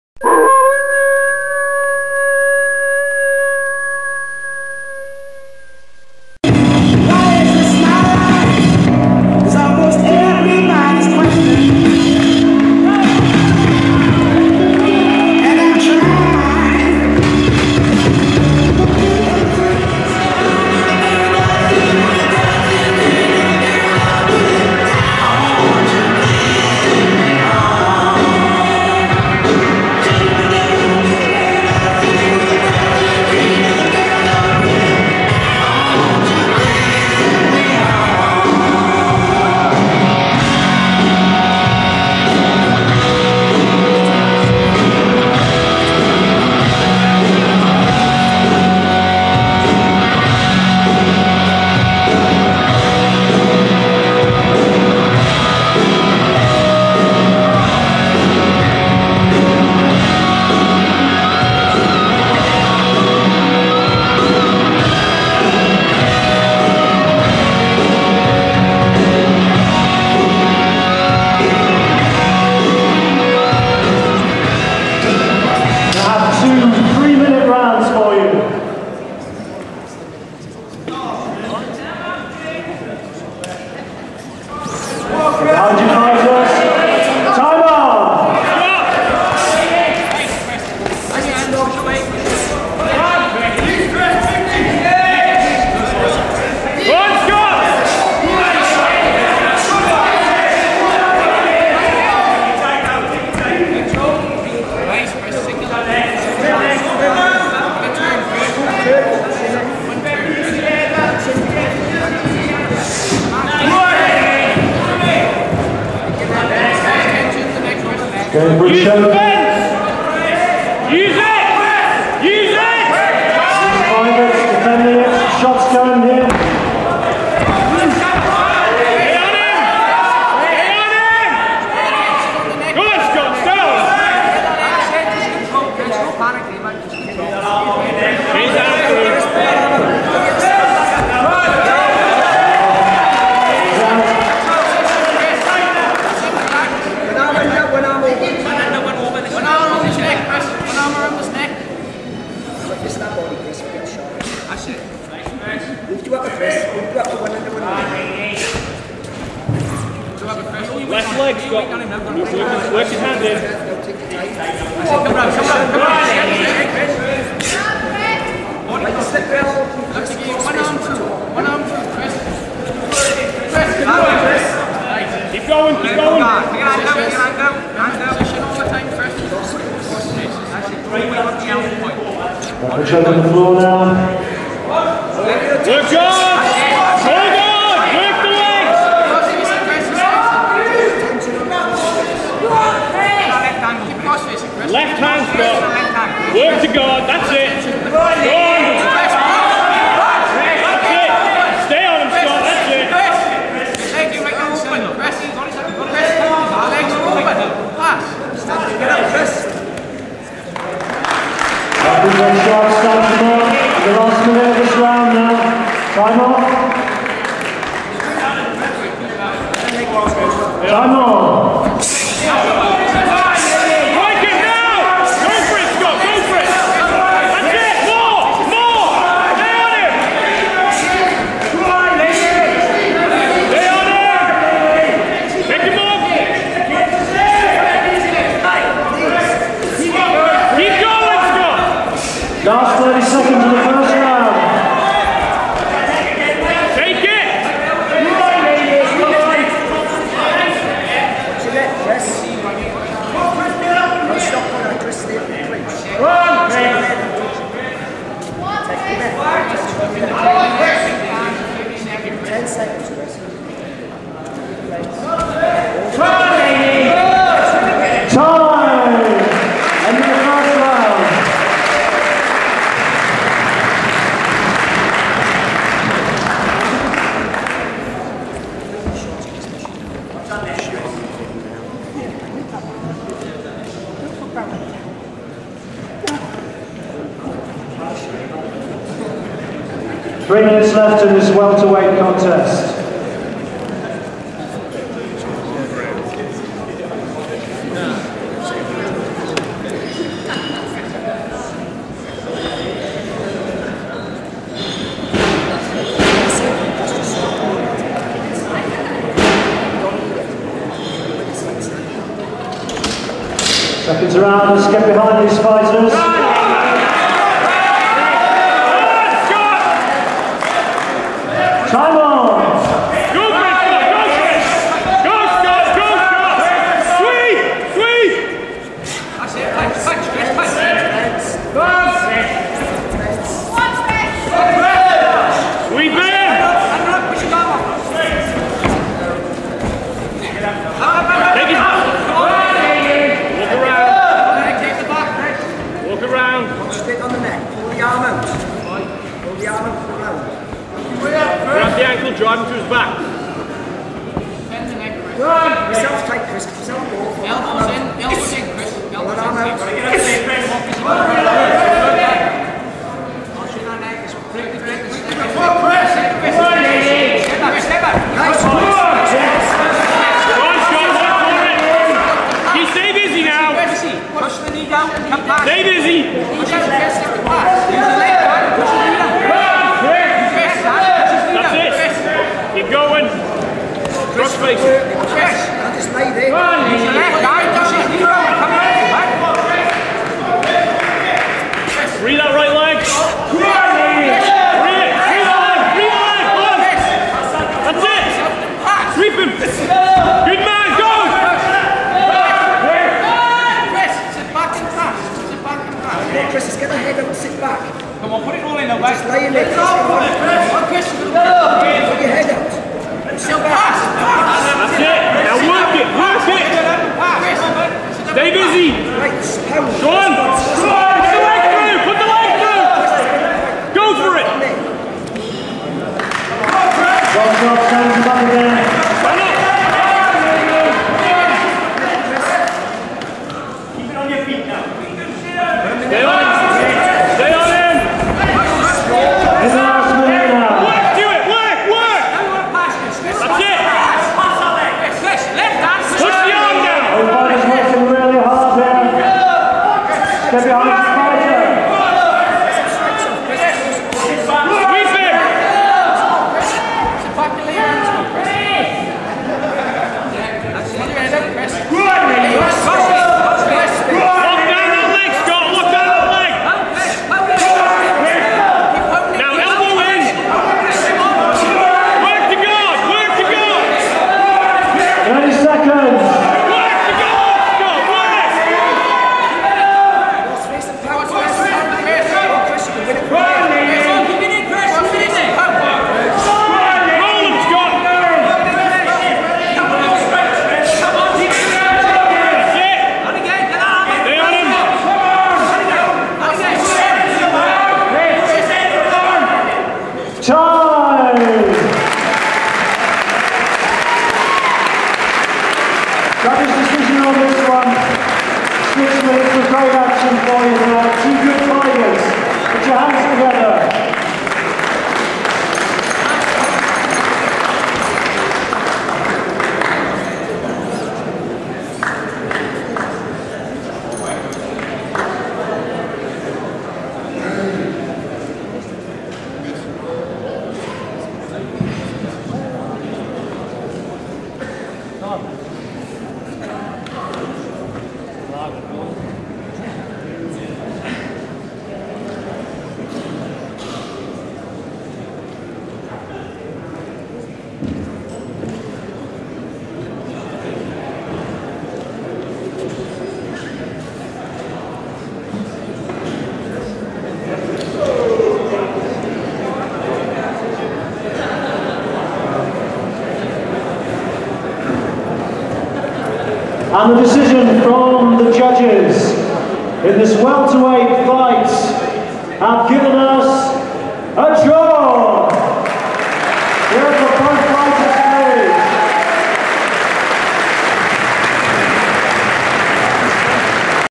Scott Newbolt G&S 13 blackdogmma
Scott fights in the cage on the under card of a semi pro event. the fight ends in a draw, one round each. plenty of submission attempts from Scott,which was  not helped by early stand ups from the ref.